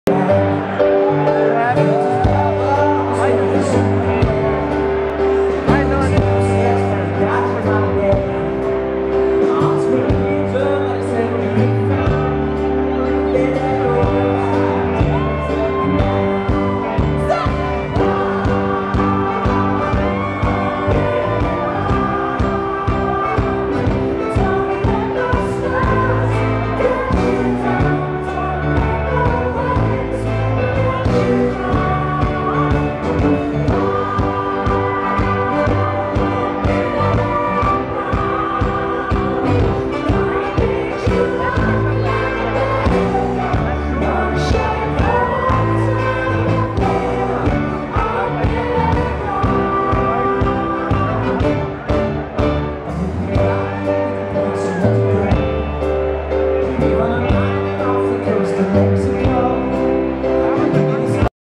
Mika
HRC Human Rights Campaign, National Dinner in Washington, D.C. USA

The Human Rights Campaign (HRC) is the largest LGBT equality-rights advocacy group and political lobbying organization in the United States.

Walter E Washington Convention Center

Washington, D.C., USA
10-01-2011